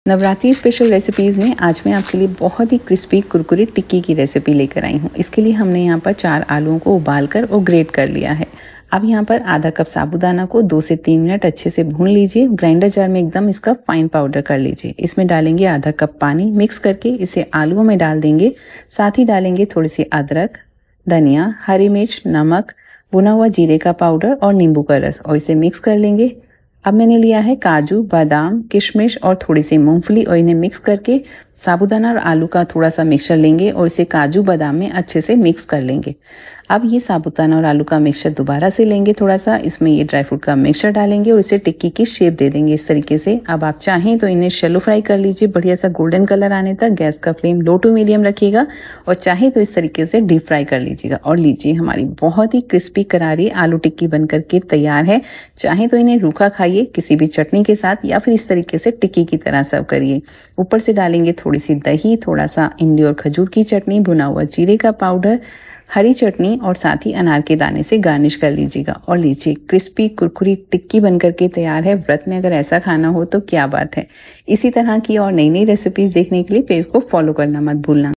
Vrat Wali Aloo Tikki: Fasting Ke Liye Perfect Snack Recipe
Ingredients-
4 Boiled Potatoes
1/2 Cup Sago pearls
1 tsp Grated Ginger
Some Coriander Leaves
1 Green Chilli
Salt to Taste
1/2 tsp Roasted Cumin Powder
1/2 Lemon Juice
1 tbsp Almonds
1 tbsp Cashews
1 tbsp Raisins
2 tbsp Roasted peanuts